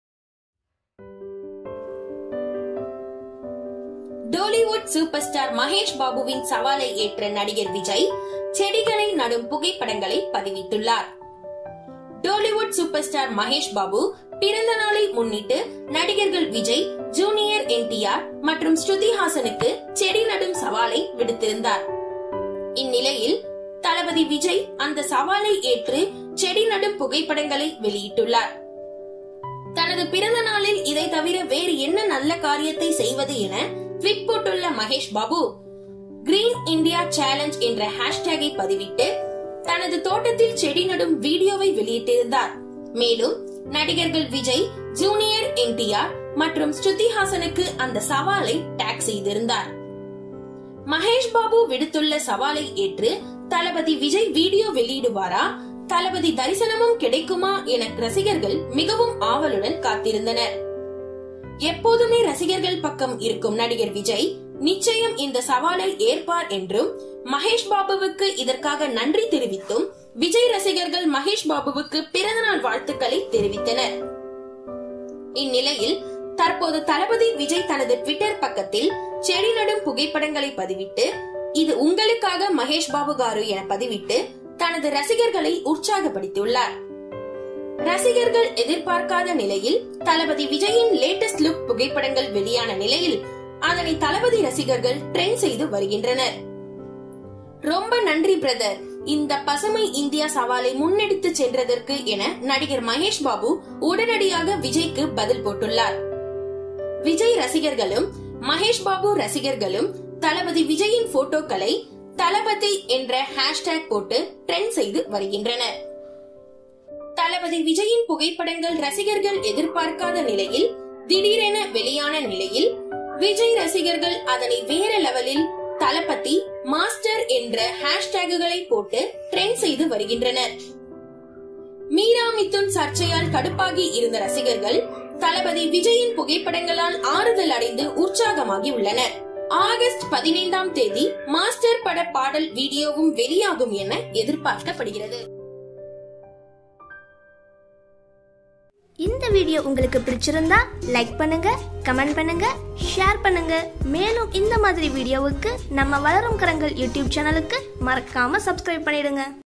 Thalapathy Vijay Accepts Mahesh Babu Challenge | இது உனக்காக நண்பா Mahesh Babu | Master Update
டோலிவுட் சூப்பர்ஸ்டார் மகேஷ் பாபுவின் சவாலை ஏற்ற நடிகர் விஜய், செடிகளை நடும் புகைப்படங்களை பதிவிட்டுள்ளார்.

டோலிவுட் சூப்பர்ஸ்டார் மகேஷ் பாபு, பிறந்தநாளை முன்னிட்டு நடிகர்கள் விஜய், ஜூனியர் என்.டி.ஆர் மற்றும் ஸ்ருதி ஹாசனுக்கு செடி நடும் சவாலை விடுத்திருந்தார்.

இந்நிலையில், தளபதி விஜய் அந்த சவாலை ஏற்று செடி நடும் புகைப்படங்களை வெளியிட்டுள்ளார்.
Thanks to Voice Over Artist : Soundarya